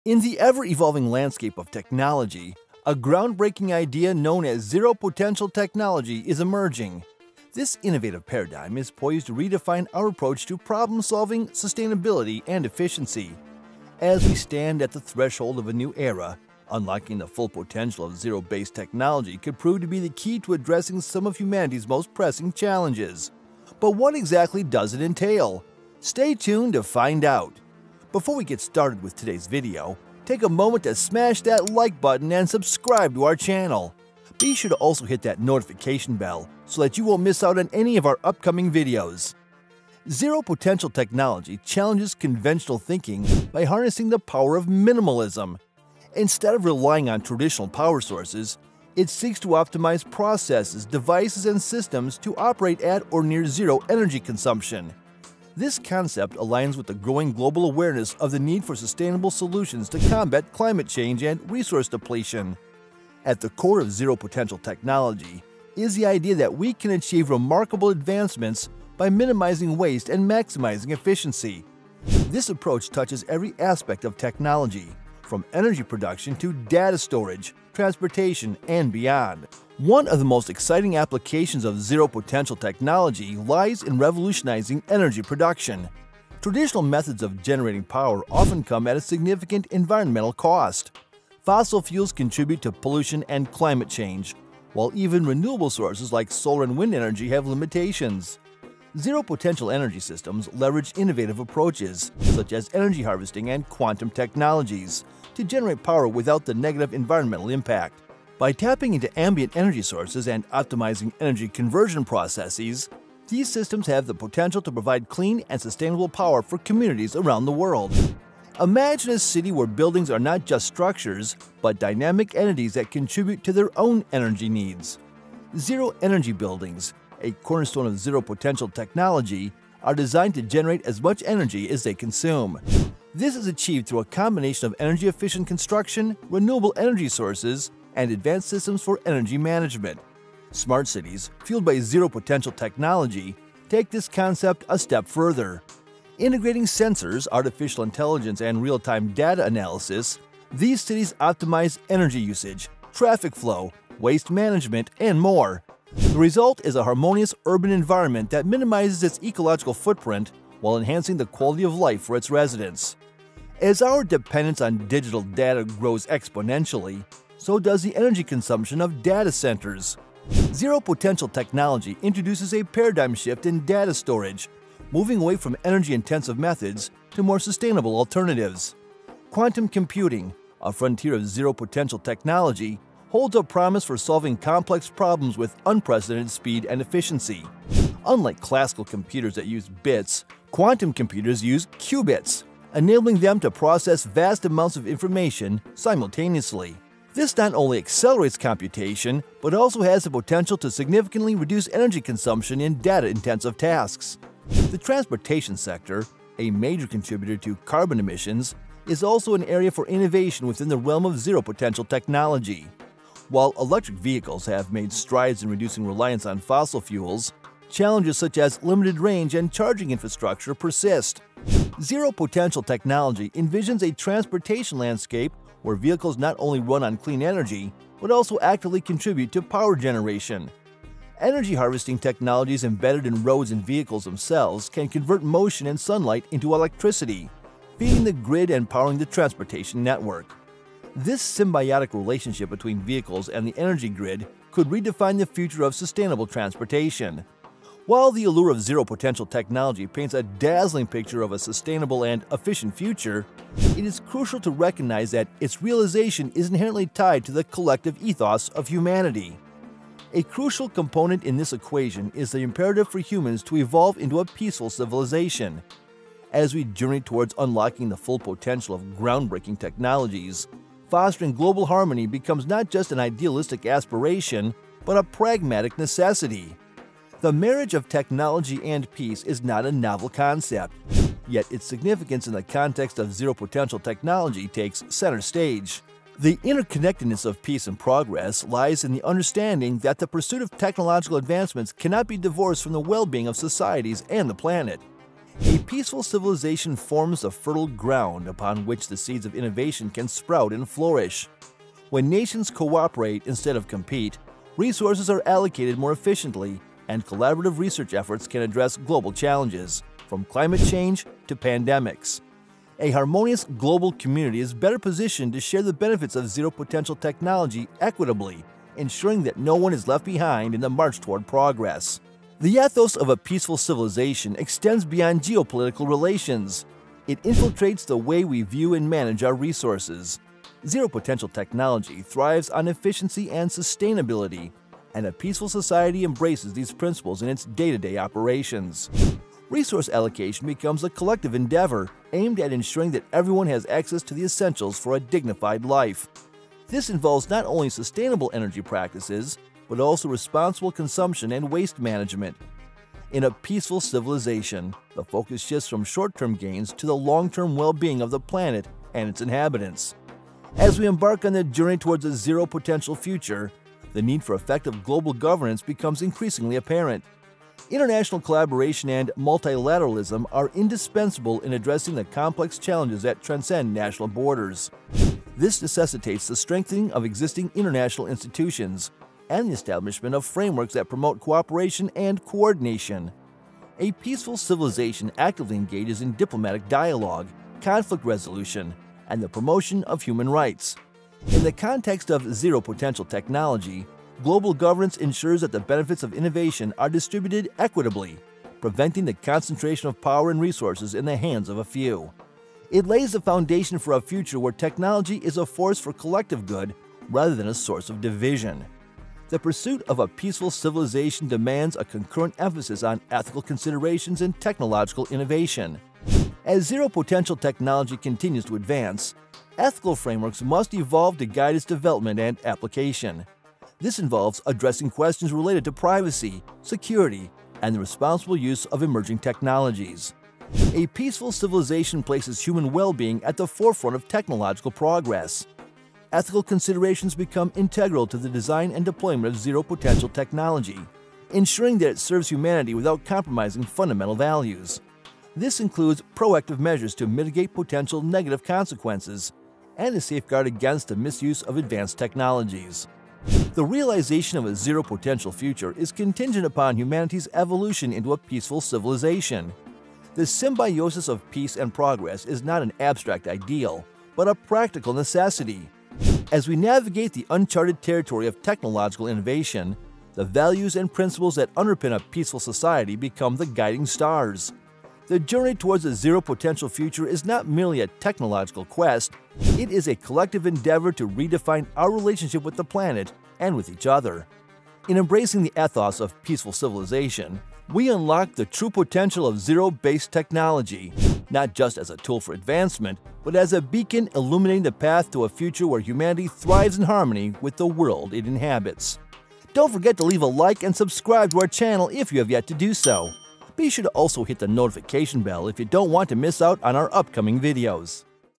Unlocking Zero Potential Technology For Our Future
In the ever-evolving landscape of technology, a groundbreaking idea known as "zero potential technology" is emerging. This innovative paradigm is poised to redefine our approach to problem-solving, sustainability, and efficiency. As we stand at the threshold of a new era, unlocking the full potential of zero-based technology could prove to be the key to addressing some of humanity's most pressing challenges. But what exactly does it entail?